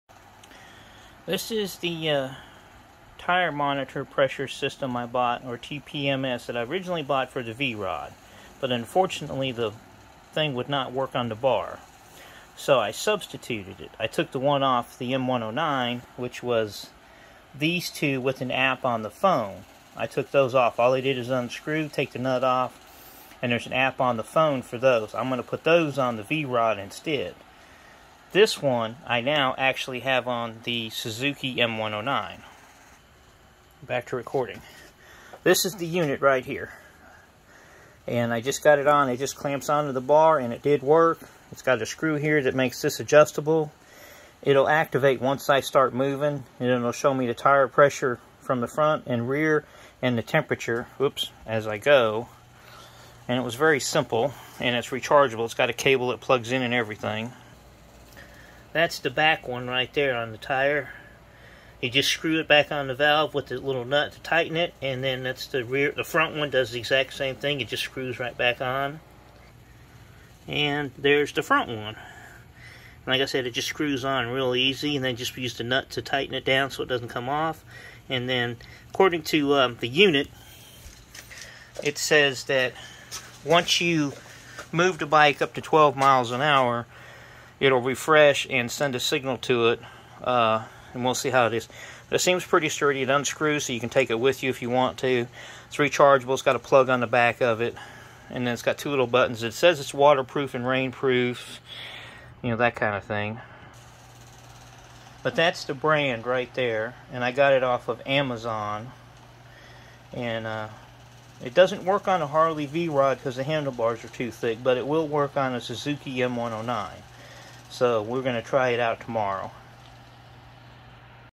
TPMS for the Suzuki m109r
A new product for Tire Pressure as I ride.  Checks the tire air Pressure and temperature too. Bought off of Amazon.